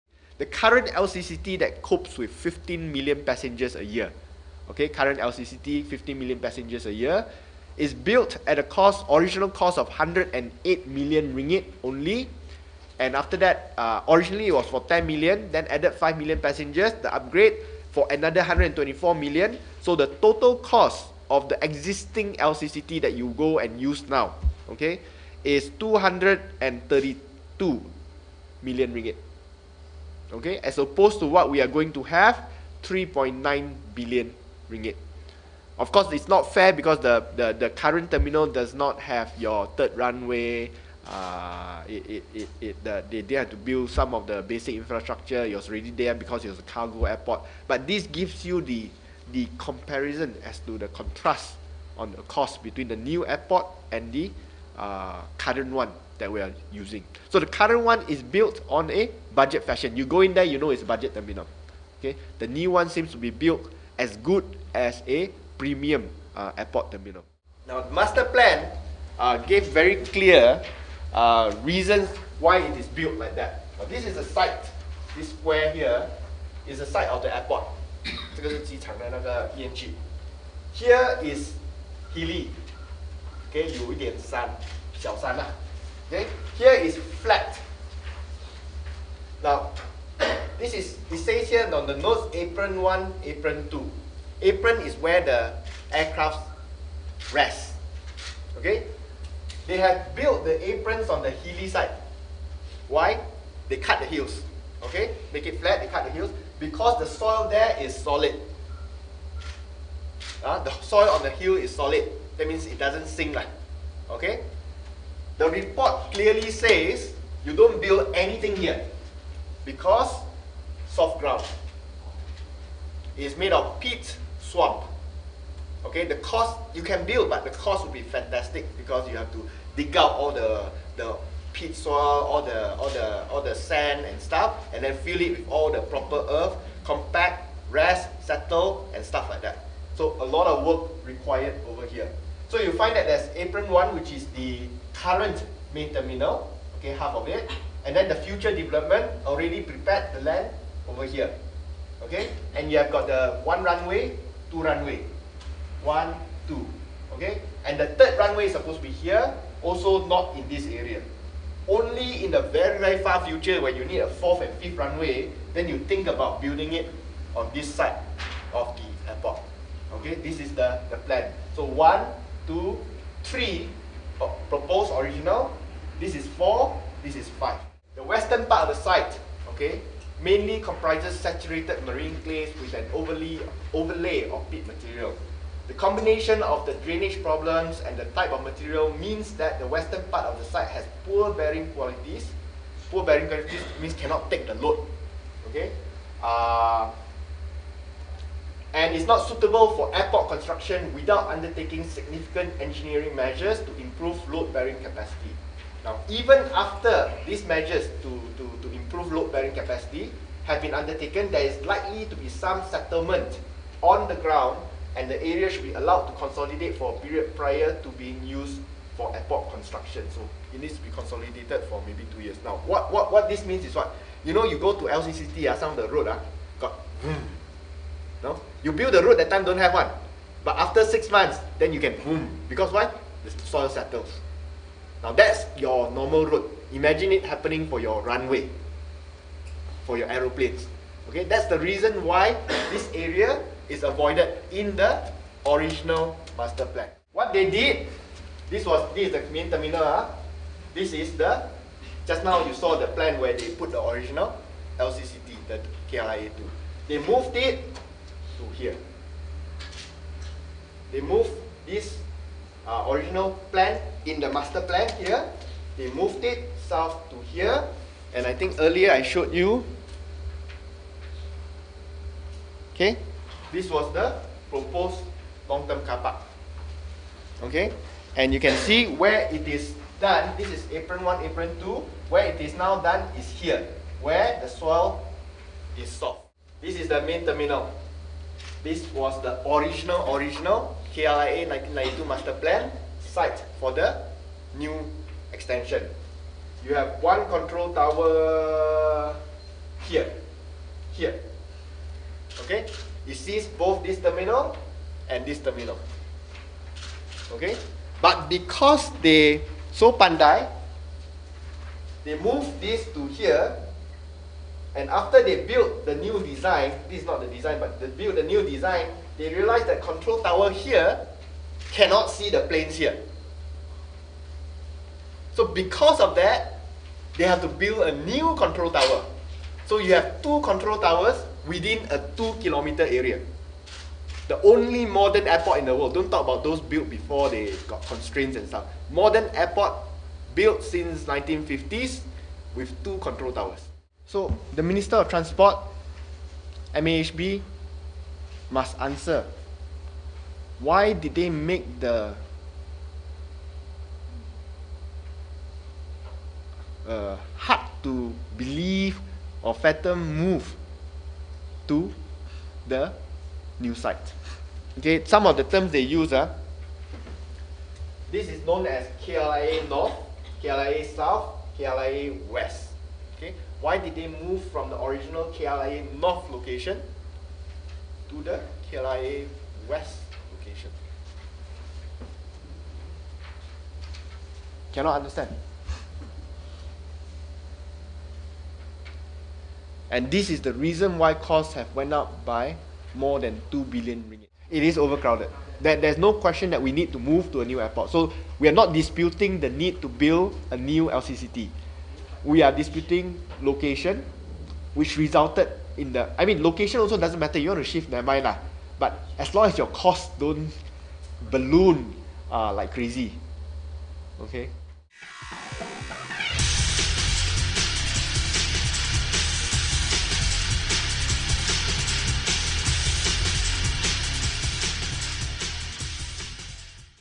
Why was KLIA2 moved to a soft soil site?
Why was a new masterplan drawn up in 2008 for the expansion of the low-cost carrier airport (KLIA2) resulting in the airport being built on soft soil, when an earlier plan circumvented this problem?

This question lies at the heart of the current outcry over the ballooning cost of the airport from an estimated RM1.7 billion to up to RM3.6 billion today.

Referring to the KL International Airport Masterplan 1992, Petaling Jaya Utara MP Tony Pua today said that the move also remains the main cause for the delays from the earlier construction deadline of September 2011 to April 2013.

Camera/Editing: Tan Jiun Wuu